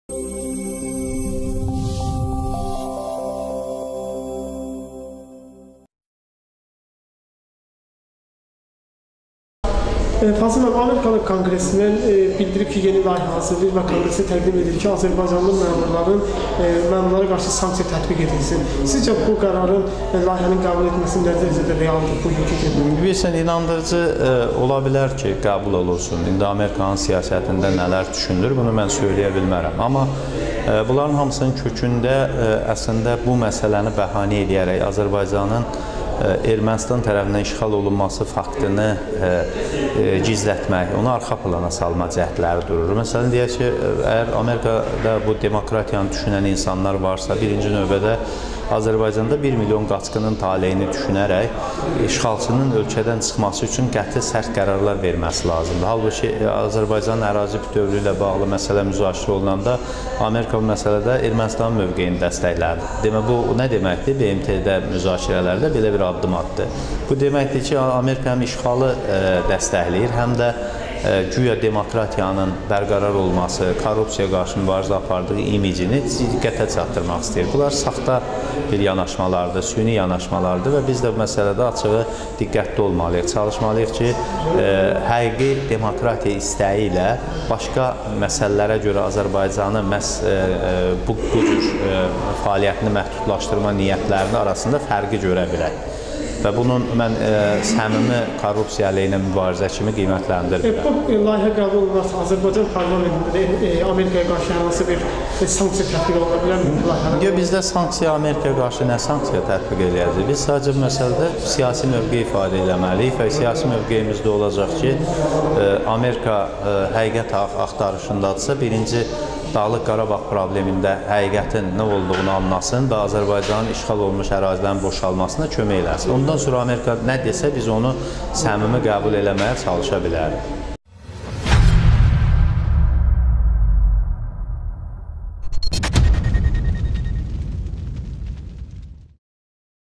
"Amerika həqiqət axtarırsa erməniyə dəstək olmasın,Qarabağ problemini araşdırsın"-Fazil Mustafa
Millət vəkili Fazil Mustafa:"Amerika siyasətində nələr düşünülür bilmirəm, amma inandırıcıdır ki, Amerika Azərbaycan siyasətçilərinə qarşı sanksiya qoya bilər. Bu məsələlərin ətrafında Ermənistanın işğalçılıq siyasətini gizlətmək durur. Amerika guya korrupsiyaya qarşı mübarizə apardığını və demokratiya yaratmaq istədiyini göstərmək istəyir ancaq işğalı dəstəkləyir. Biz bu məsələlərdə diqqətli olmalıyıq.
Biz Amerikaya qarşı heç bir snaksiya tətbiq edə bilmərik , biz sadəcə siyası mövqe ortalığa qoymalıyıq. Amerika həqiqət axtarışındadırsa ilk olaraq "Dağlıq Qarabağ" problemindəki həqiqəti araşdırsın və Azərbaycan işğal altında olan ərazilərin boşalmasına kömək etsin!"